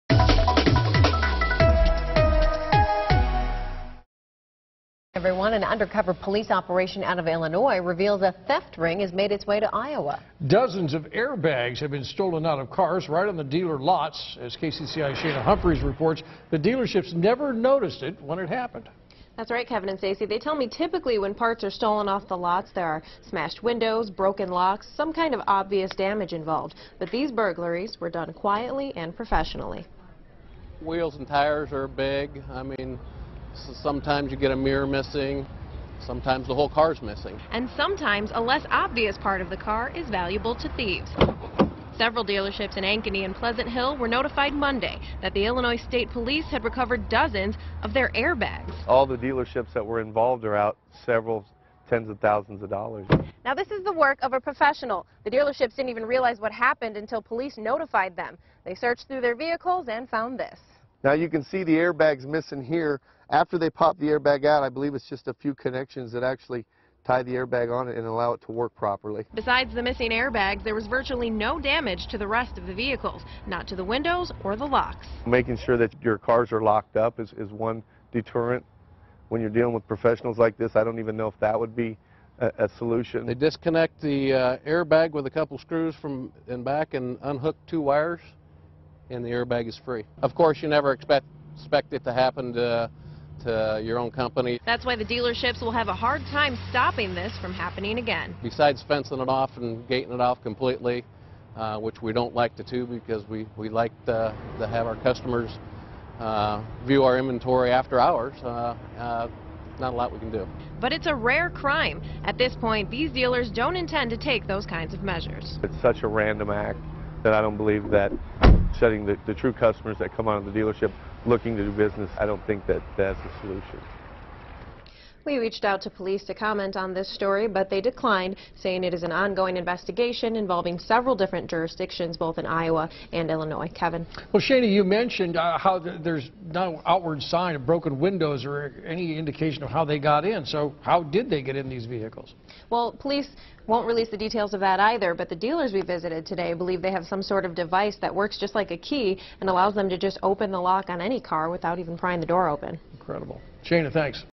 Thieves steal air bags from cars at dealerships
Air bags are stolen from dozens of new cars at metro dealerships.